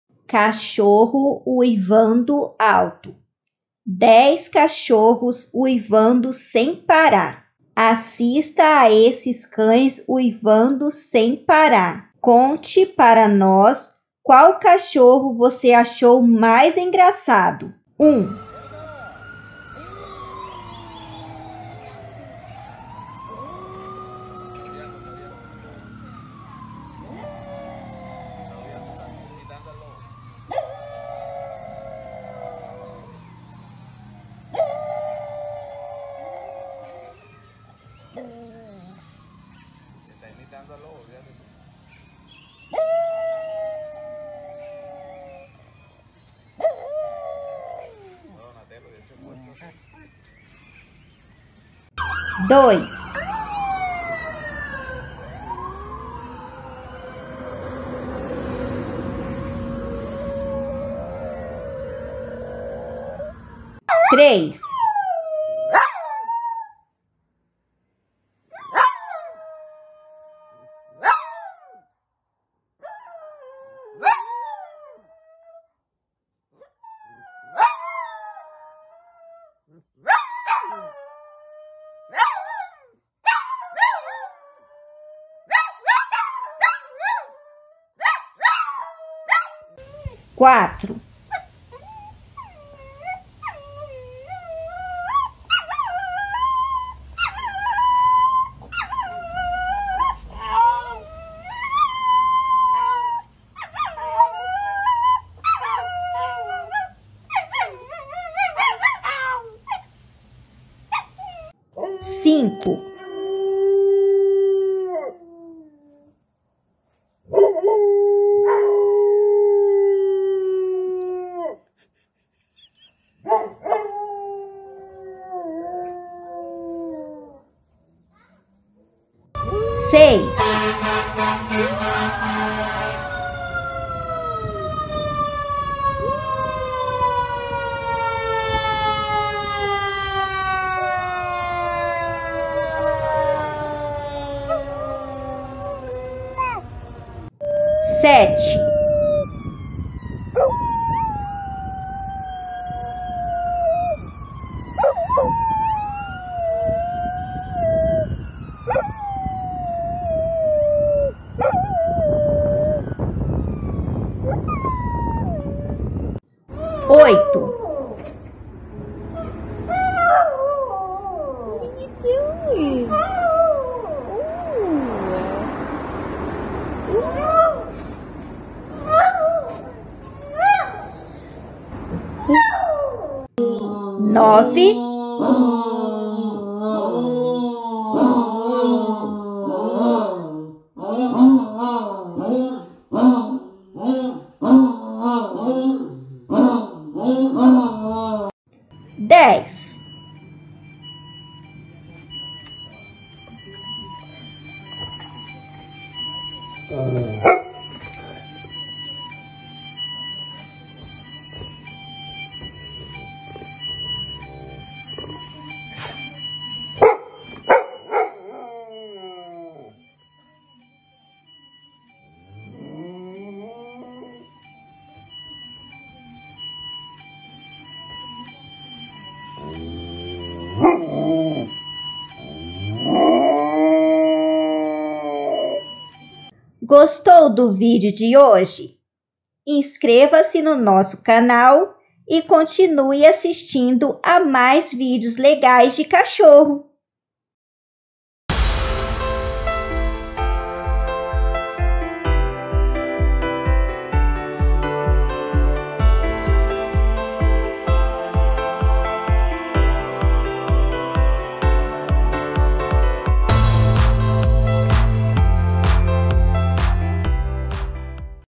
Cachorro Uivando Alto - 10 Cachorros Uivando sem Parar [Auuuuu, Uuuuuu] 🎶 🐶
Neste vídeo você assistirá a 10 cachorros diferentes uivando alto sem parar para você apresentar ao seu cão.

Mostre o som de uivos de cachorros ao seu cão e nos conte como ele reagiu ao efeito sonoro. Trole o seu pet, seja ele cão ou gato, e comente abaixo como ele se comportou ao ouvir cachorros uivando alto.

E o seu dog, ele começou a uivar também?

Você sabe por que o cães uivam?

É um comportamento dos cachorros principalmente para se comunicarem com outros cães à distância.

Basicamente é uma forma de comunicação do dog, mas que possui diversos motivos, como alerta de perigo, fome ou sede, ansiedade por separação, presença da cadela no cio, dor.

Outra face do comportamento social do cão é associação. Quando um cão late, os outros que estão próximos tendem a latir também. Da mesma forma ocorre em relação ao uivo. Se um cachorro uivar, os outros também uivarão, como uma forma de imitação.

Vamos a um exemplo: o cão acende o alerta de perigo quando ouve barulhos de sirenes de ambulância ou instrumento musical.

Neste vídeo você mostramos algumas situações citadas acima, de cães uivando alto ao ouvir barulhos de sirenes, ansiedade por separação.

Qual a diferença entre latido e uivo?

O uivo de cachorro atinge um volume alto e duração contínua. Já os latidos são curtos e contínuos, às vezes.
De forma gráfica, verifique abaixo como é um pouco o uivo e latido de cachorro:
☑️Latido: au, au, au, au
☑️Uivo: auuuuuuu, uuuuuuuu

Para que consiga perceber a diferença entre os uivos e latidos de cães, temos no nosso canal vídeos sobre cães latindo. Assista a esses vídeos também e entenda como é o latido e uivo de cachorro.

Temos uma seleção de 10 cães uivando alto. Alguns são engraçados, outros já uivam de forma estridente, uns mais graves, ou seja, cada cão uiva da sua maneira, tentando se comunicar com outros cachorros ou até seres humanos.

É importante você ficar atento (a) como, por qual motivo, e frequência dos uivos de seu cachorro, pois há várias causas para exteriorizar esse comportamento.

Pode ser que ele esteja precisando de mais companhia e atenção, como levá-lo para passear e brincar um pouco com seu animalzinho de estimação.